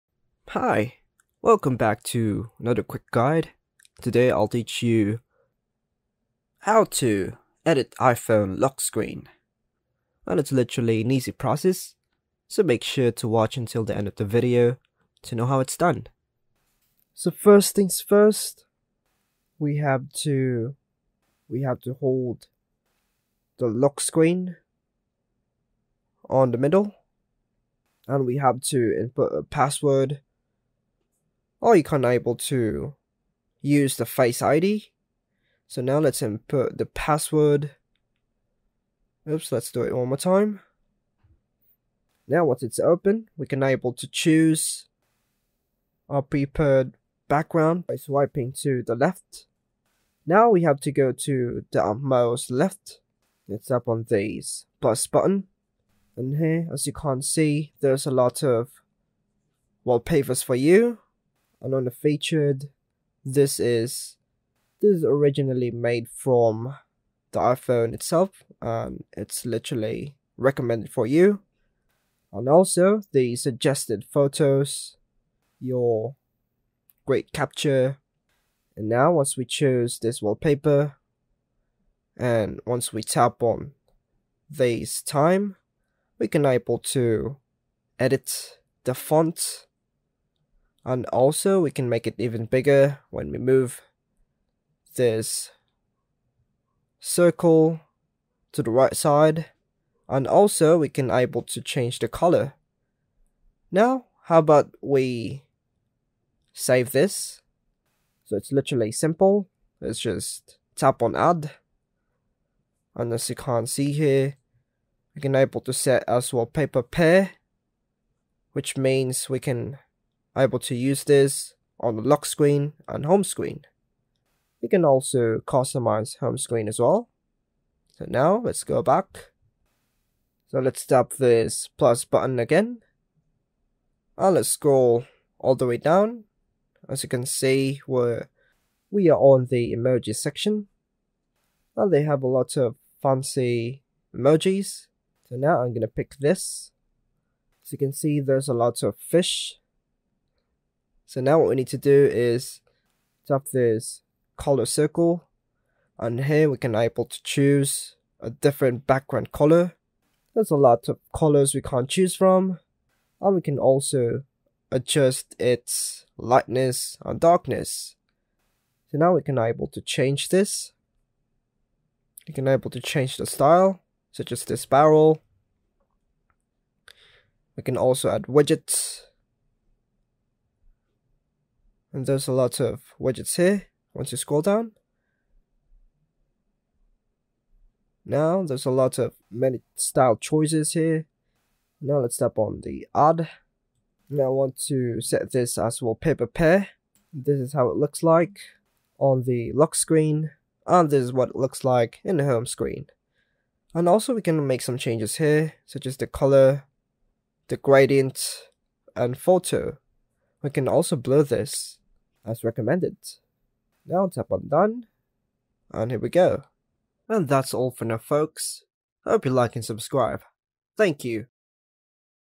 How To Edit iPhone Lock Screen (Quick & Easy)
This lesson will teach you how to edit iPhone lock screen widgets and general look. You can customize a lot about the iPhone lock screen, which we discuss in the video. This guide also covers how to change iPhone lock screen wallpaper.